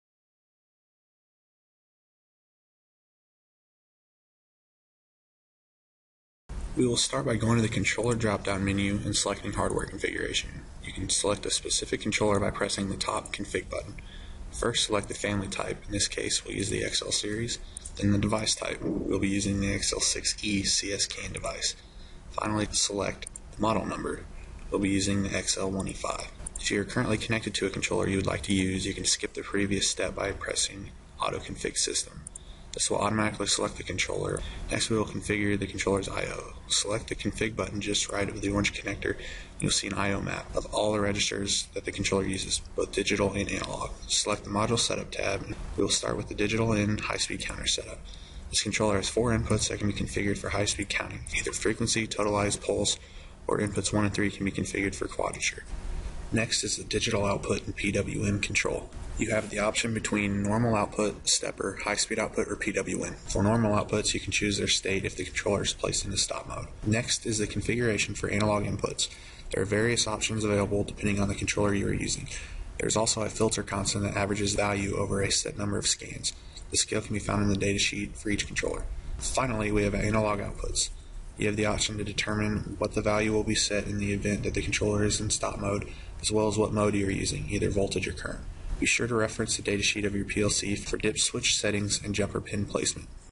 HARDWARE CONFIGURATION Basic Setup
We will start by going to the Controller drop down menu and selecting Hardware Configuration.
We can select a specific controller by pressing the top Config button.
First select the family type.  In this case we will stick with the XL Series.
Then select the device type.  We will be using the XL6e CsCan device.
Finally Select the Model number.  We will be using the XL1E5.
If you are currently connected to the controller you would like to use, you can skip the previous step by selecting Auto Config System.  This will automatically select the controller that you are connected to, although you will lose any of the settings that you have already configured.
Next we will configure the controller's I/O.  Select the Config button just right of the orange connector.  You will then see an I/O map of all of the registers that the controller uses, both digital and analog.  Select the Module Setup tab in order to change the I/O settings.
We will start with the Digital In High Speed Counter setup.  This controller has 4 inputs that can be configured for High Speed Counting.  Frequency, Totalize, Pulse, and Inputs 1 and 3 can be configured for Quadrature.  You can then chose a specific mode and Counts per revolution if available.
Next is the Digital Out and PWM control.  You have the option between Normal Output, Stepper, High Speed Output, or PWM.  For normal outputs, you can chose their state if the controller is placed into Stop mode.
Next is the configuration for Analog Inputs.  There are various options available, depending on the controller you are using.  There is also a filter constant that averages the value over a set number of scans.  The scale can be found in the datasheet for each controller.
Finally we have Analog Outputs.  You have the option to determine what the value will be in the event that the controller is in Stop Mode, as well as what mode you are using, either voltage or current.
Be sure to reference the datasheet of your PLC for dip switch settings and jumper pin placement.